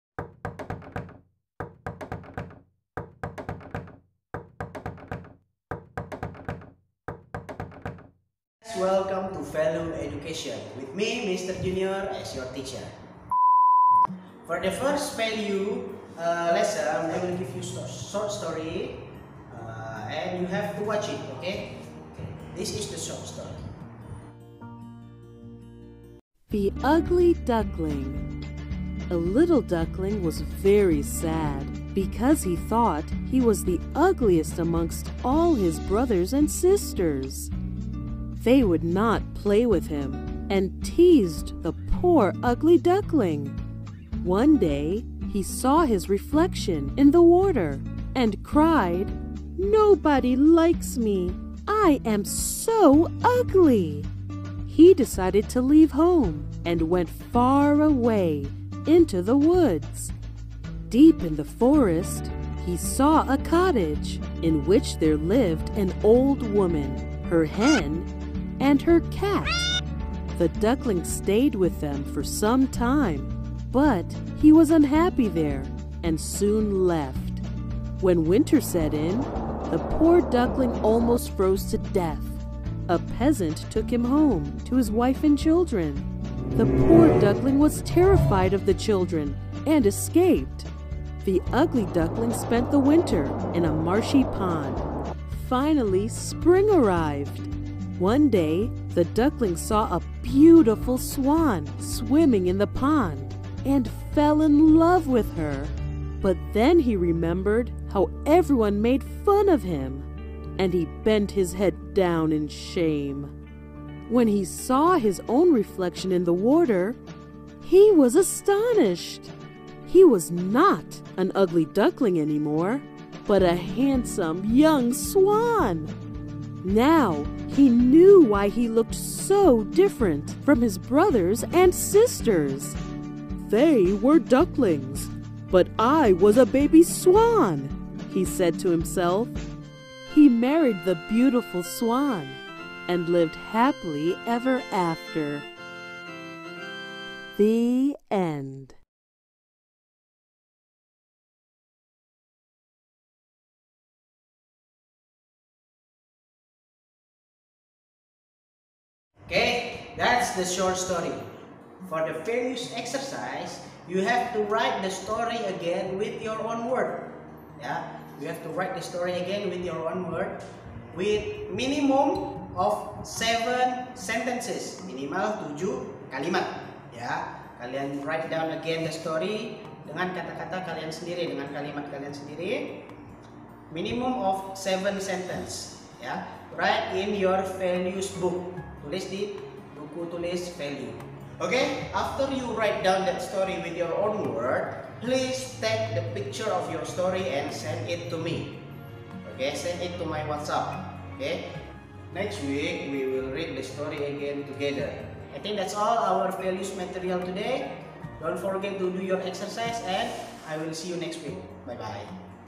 Value Education - Short Story - 5th Grade
This video is the materials and exercise of Value Education Subject. The subject is mainly for the 5th Grade students, and is about reciting the given short story